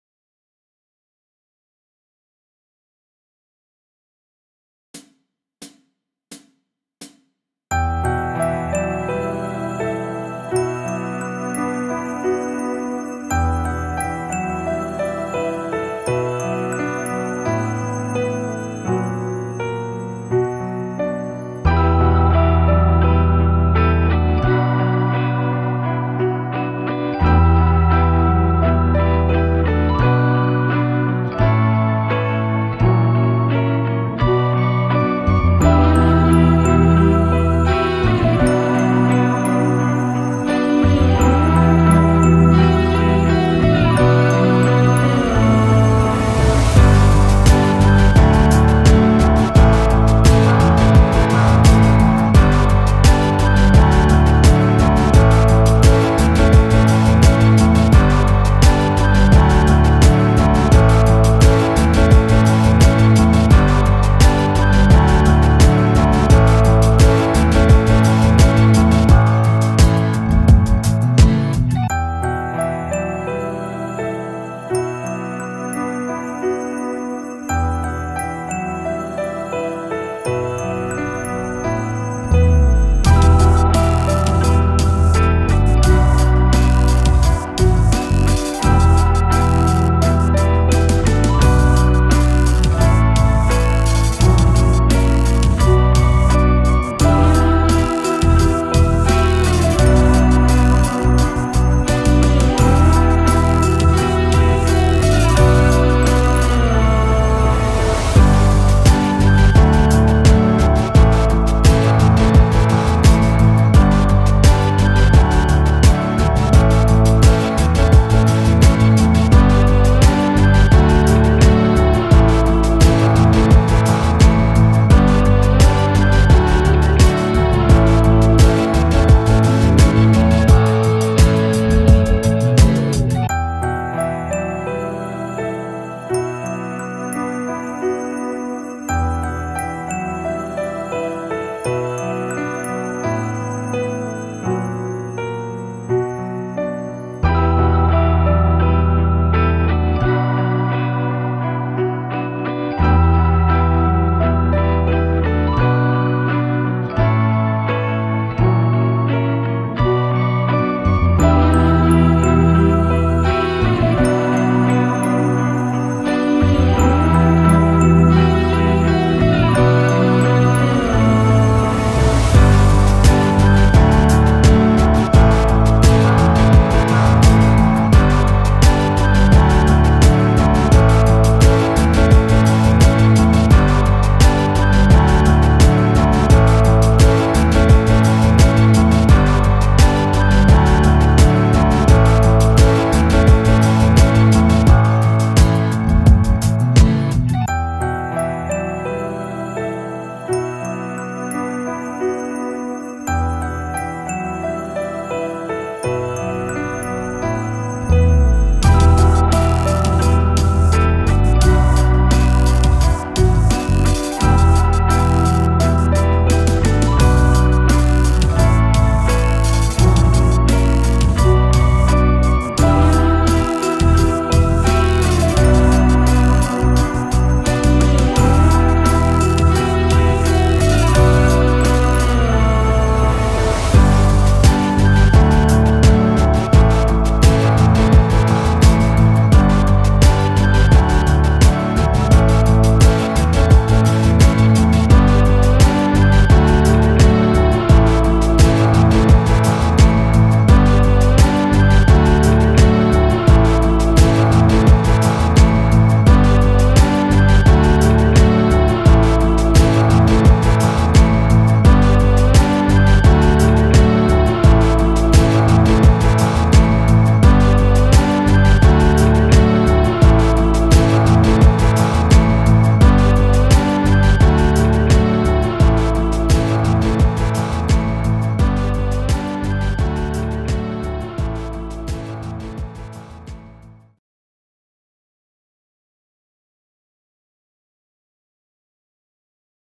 Guitar Jam Track in B Minor - Easy Pop Ballad | 86 BPM
Pop ballad backing track in B Minor, that makes it fun to jam along using common chords and progressions, and to improvise using common major/minor scales!